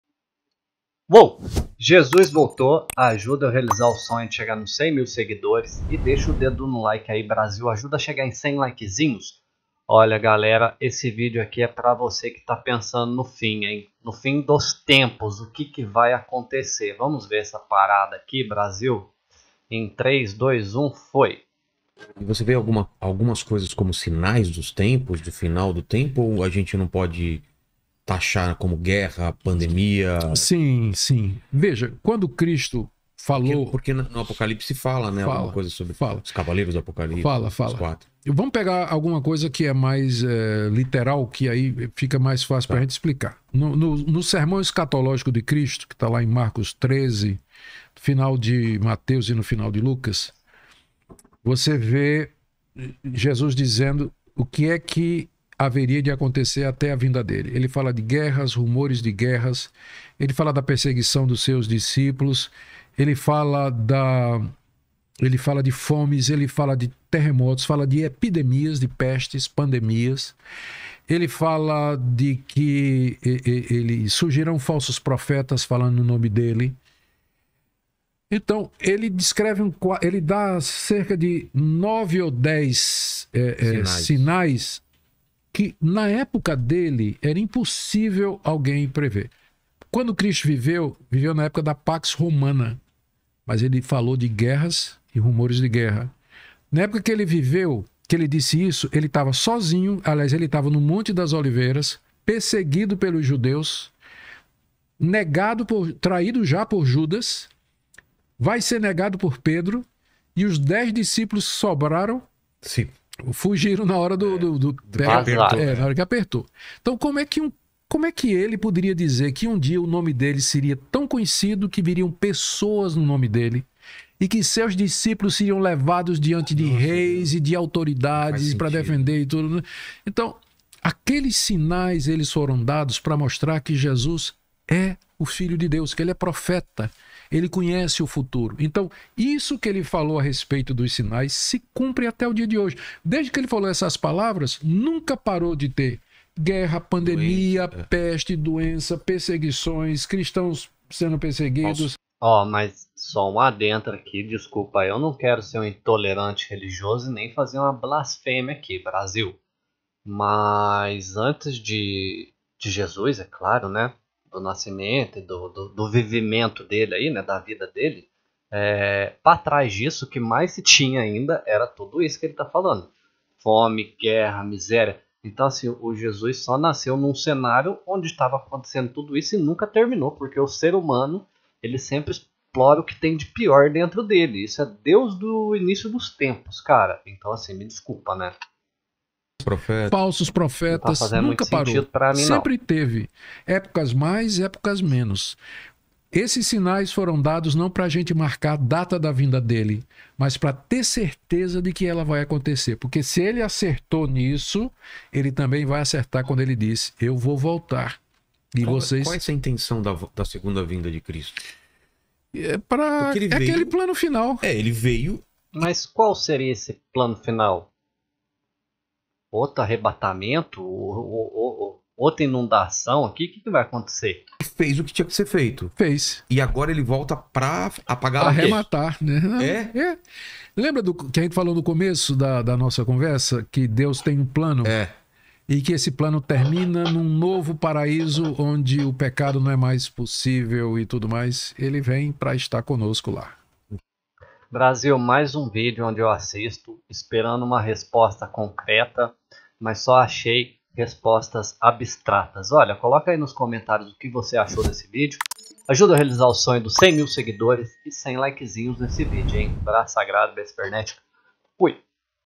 3 COISAS QUE JESUS DISSE E ESTÁ ACONTECENDO O APOCALYPSE ESTÁ PROXIMO VILELA SE ASSUSTA COM NICODEMU
AUGUSTUS NICODEMUS é pastor protestante, escritor, teólogo, professor e conferencista. Ele é considerado um dos maiores teólogos e pregadores do Brasil.

siga no istagram @jesusdastreta
tik teko @jesusdastreta